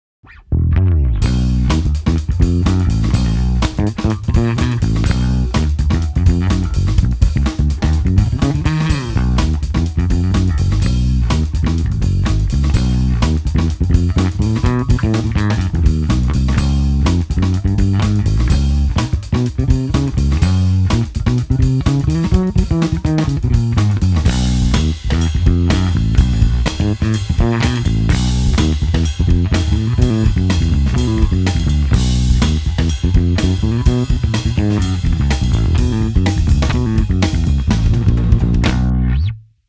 Funky Bass Groove (tribute to Jaco)
Inspired by the great Jaco Pastorius, I decided to play around on a bit of a funk-based groove in B. I tried to go for that highly dynamic, syncopated style that features a lot in Jaco's playing.

Pretty much as the title says, grabbed my Ibanez Artcore and played around with some chord melodies.

Check out loads more guitar and music related content on my